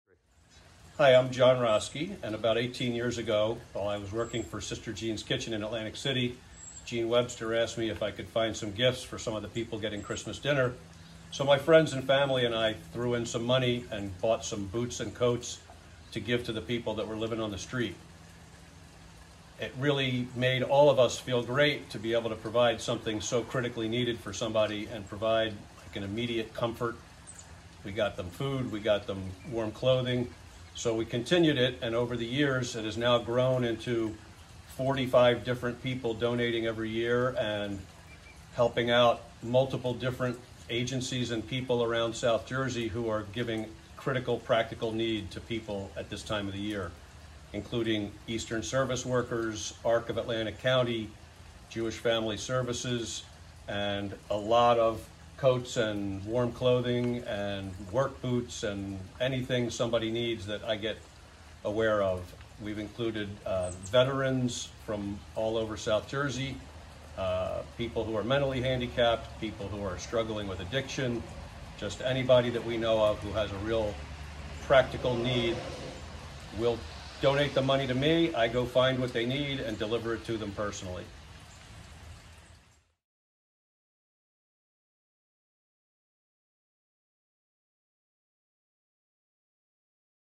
Egg Harbor Township man spends Christmas giving to the needy
Jon Rosky, 59, of Egg Harbor Township, readies a load of donations for the needy, including winter jackets and backpacks filled with cold weather gear and food. Video by Matthew Strabuk, for The Press.